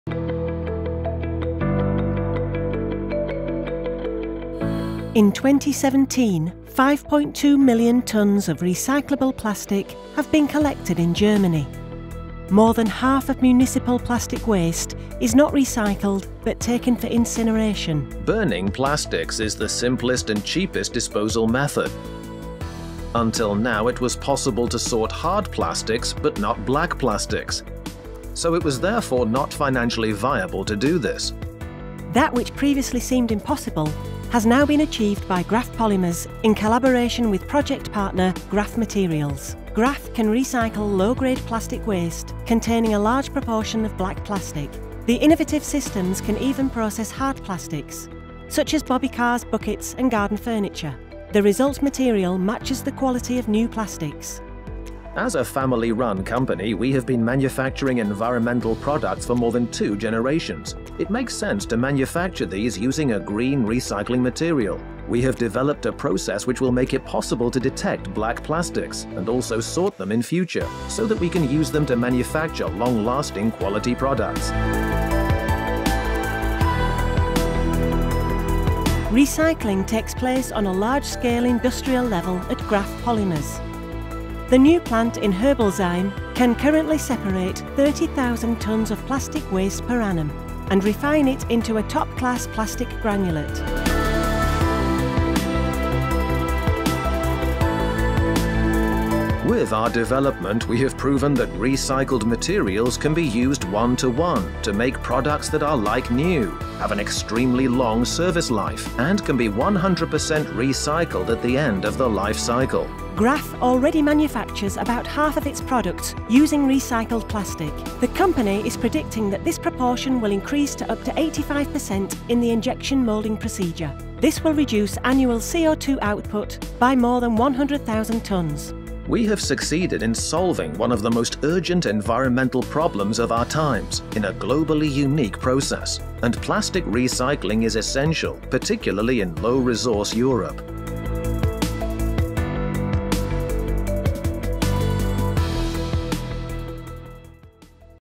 Nominated - German raw material efficiency award 2018
That which previously seemed impossible has now been achieved by Graf Polymers in collaboration with project partner Graf Materials. You can recycle low-grade plastic waste containing a large proportion of black plastic.The new plant in Herbolzheim can currently separate 30,000 tons of plastic waste per annum and refine it into a top-class plastic granulate.